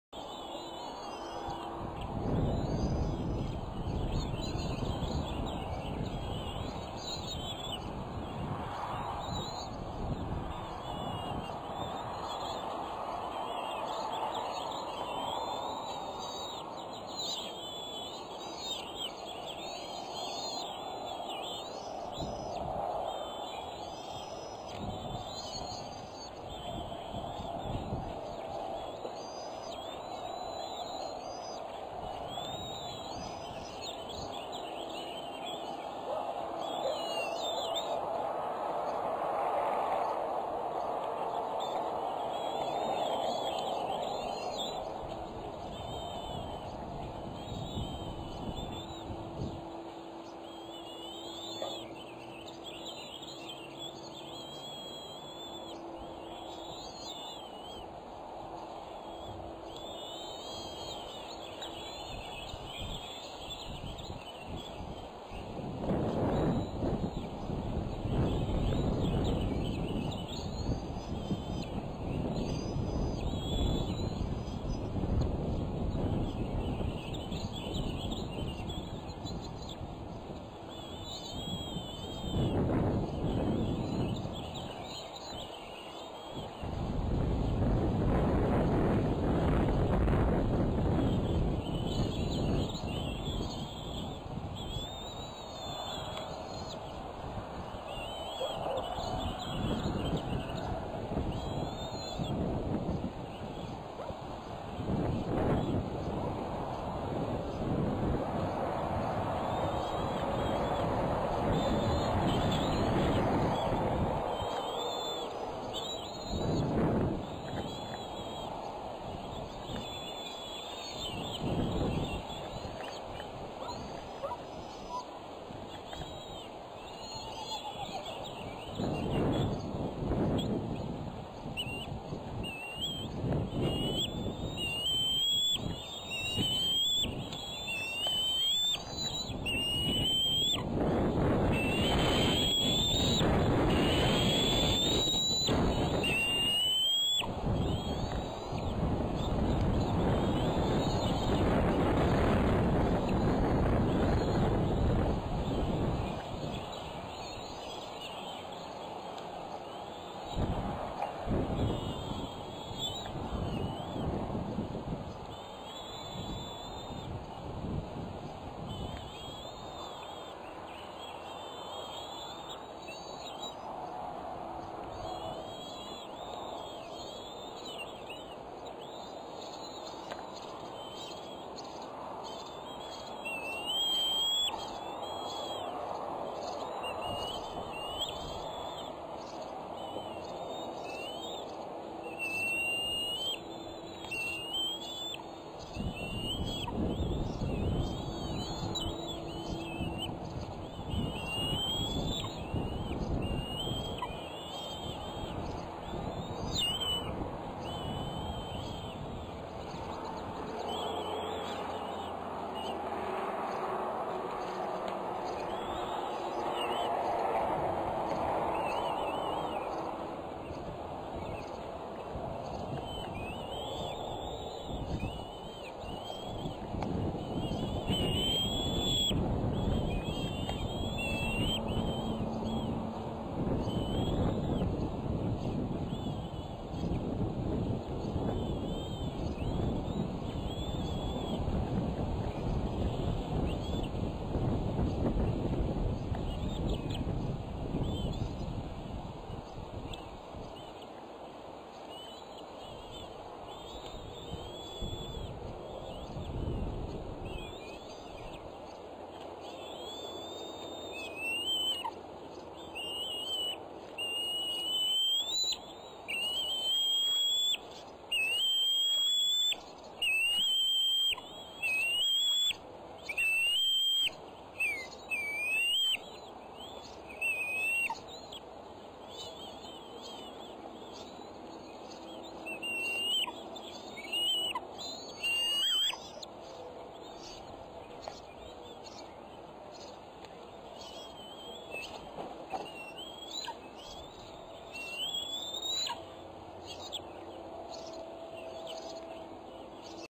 Bald Eagle Nest - Decorah, Iowa 05-11-2012; 18:57 CDT (5min)
The nest is running out of room with the five eagles (mom, dad, D12, D13, D14) sharing it at the same time.